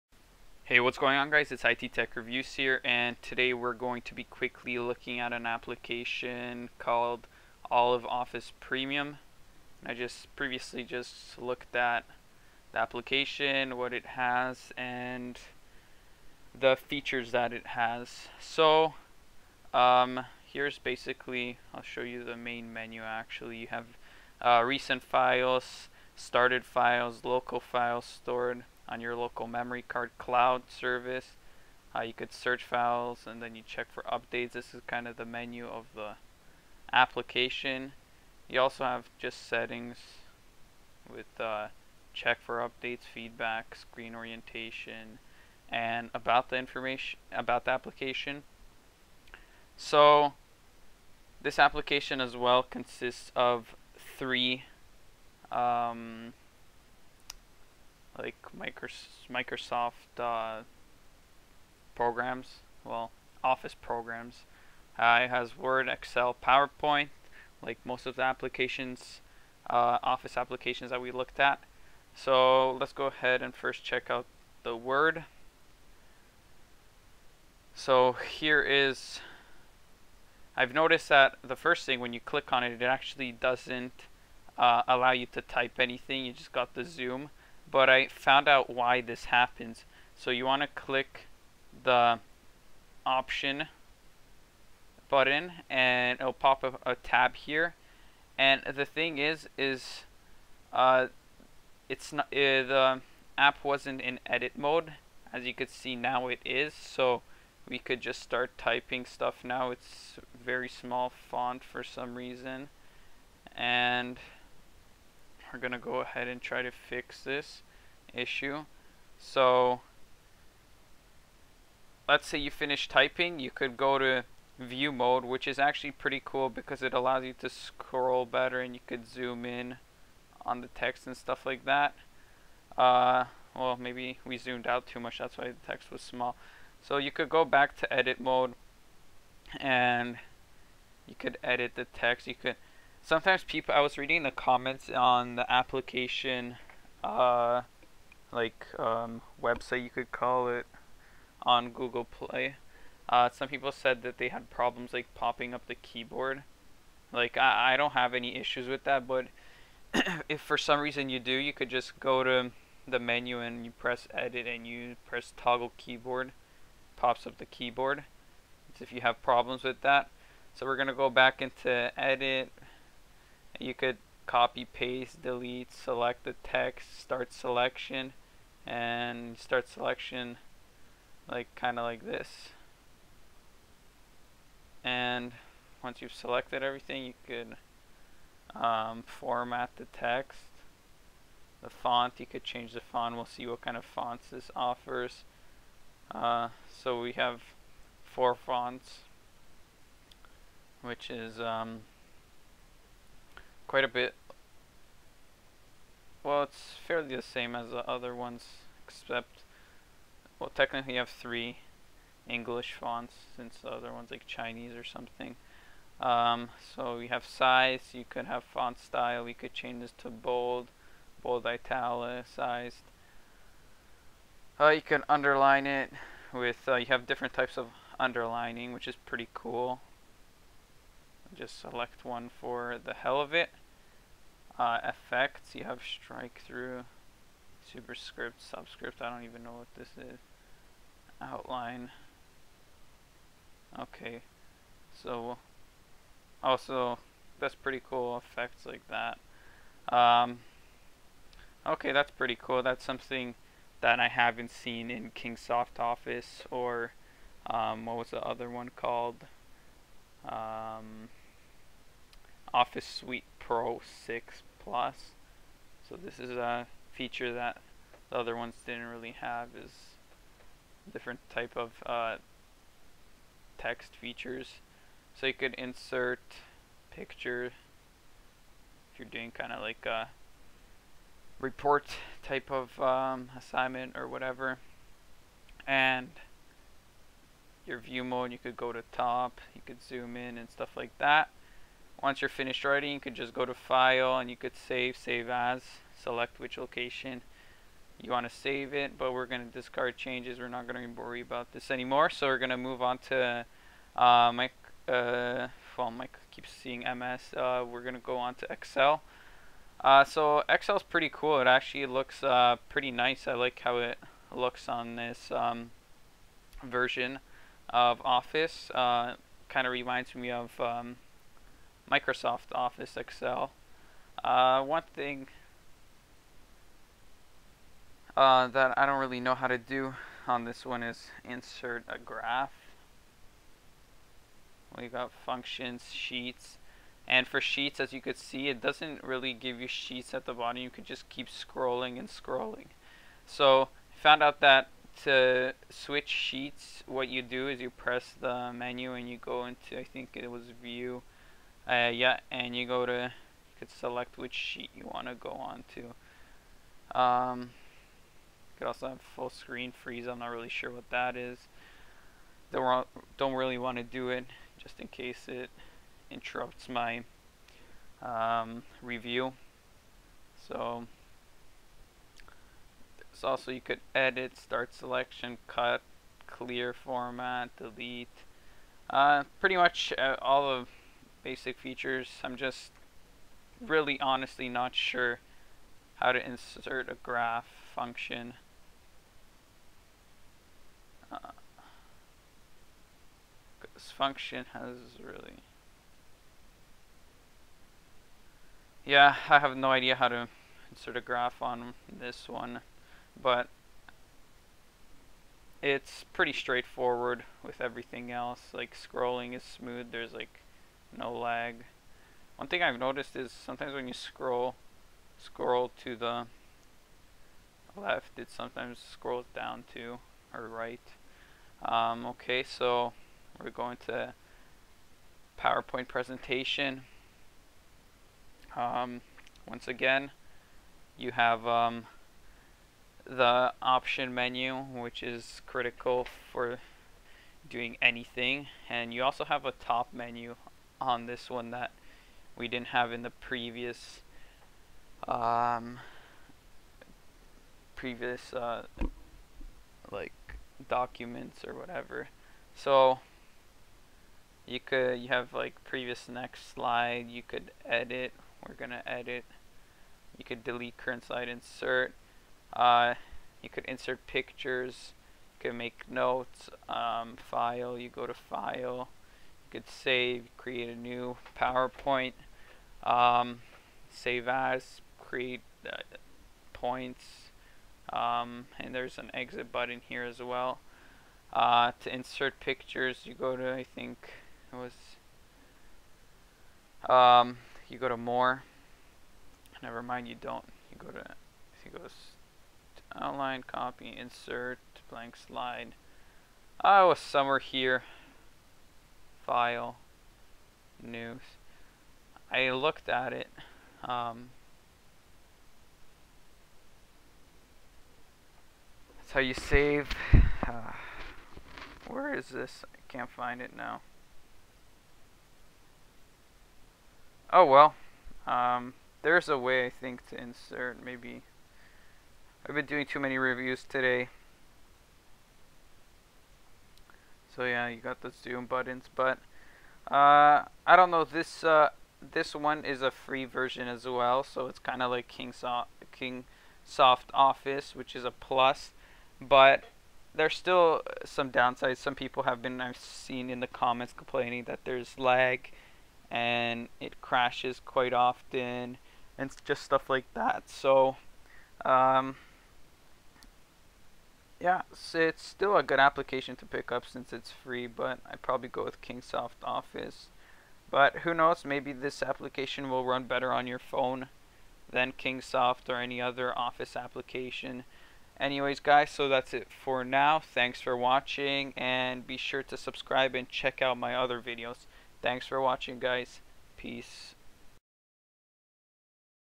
Application Review: Olive Office Premium Free (Android)
Greatly Appreciated. Quick review of Olive Office Premium Free Android Version. Great application. Want to request a Application Review?